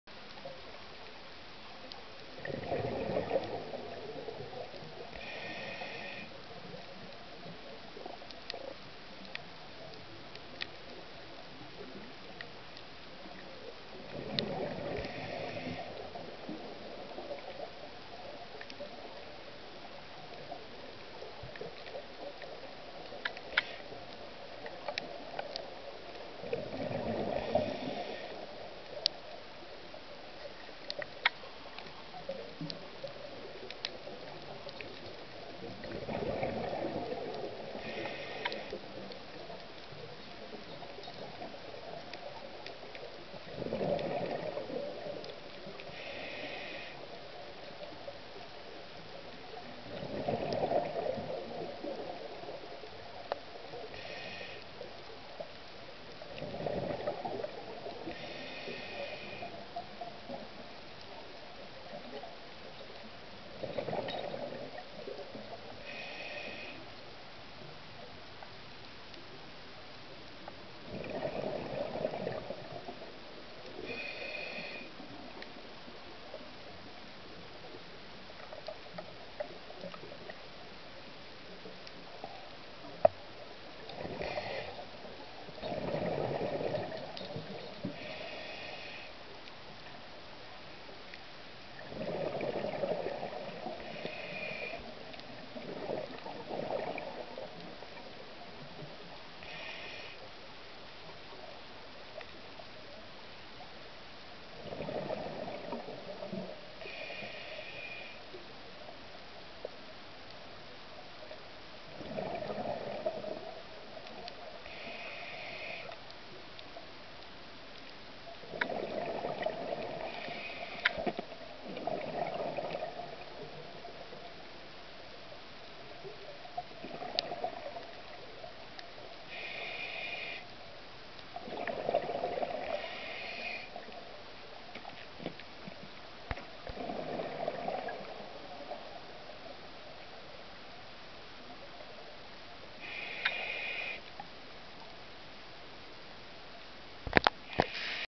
Peleliu Drop-off@PALAU
棚の上を撮っていたら、最後にグレイリーフシャークの姿が。見ると下にうようよ！